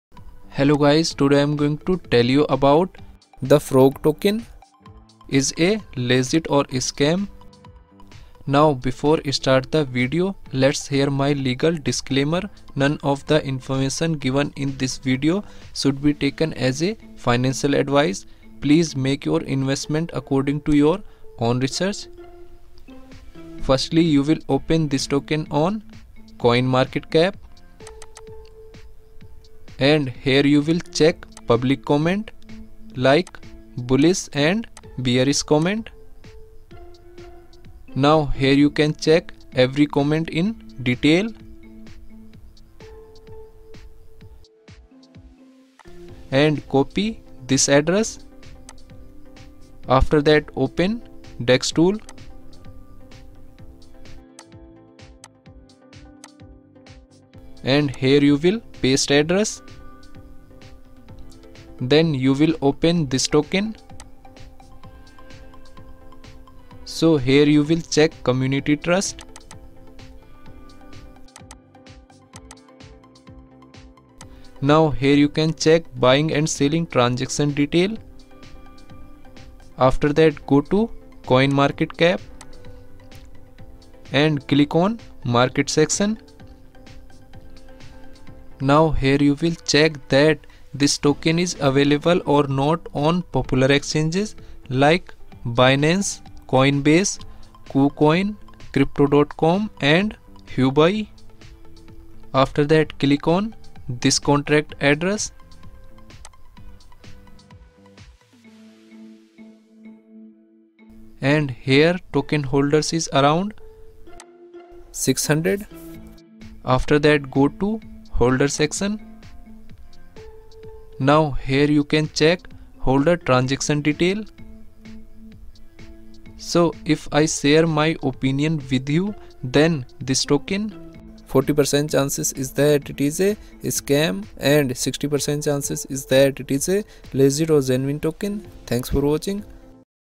Is The Frog (FROG) Token Legit or Scam ??
In this video, I am going to share that The Frog (FROG) Token is legit or a scam. I have made videos based on online data, I am not a financial advisor investing in crypto at my own risk.